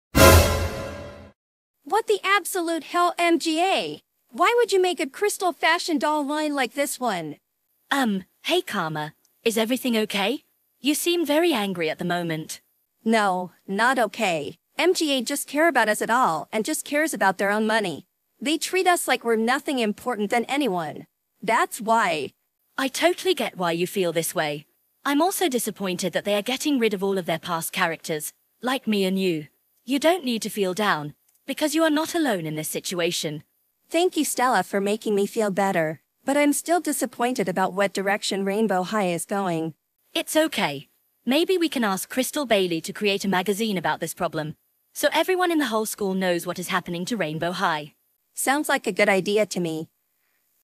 Karma Nichols' reaction to the latest RH doll leaks
Karma Nichols, the Rainbow High student known for her series "Kontent With Karma", was very crestfallen after she saw the leaks of the latest Rainbow High crystal fashion dolls on social media. Stella Monroe checks on her to see if she's alright, as she cheers her up from this situation. At the end, both had the idea to ask Krystal Bailey to make a magazine about the rebrand so all the students of Rainbow High can remember this.
All characters' voices are cloned on weights.gg